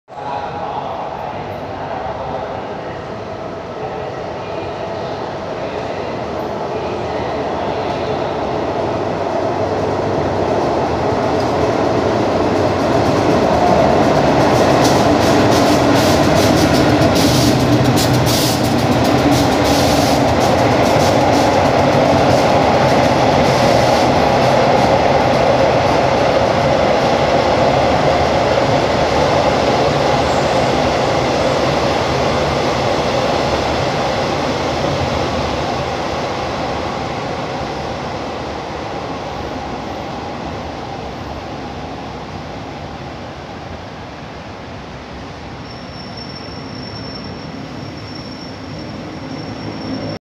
ขบวน 207 GEA.4542 ถึงดอนเมือง
GEA.4542 ทำขบวน 207 (กรุงเทพ - นครสวรรค์) ถึงดอนเมือง เวลา 14.54 น. ตามเวลา
22 มิ.ย.66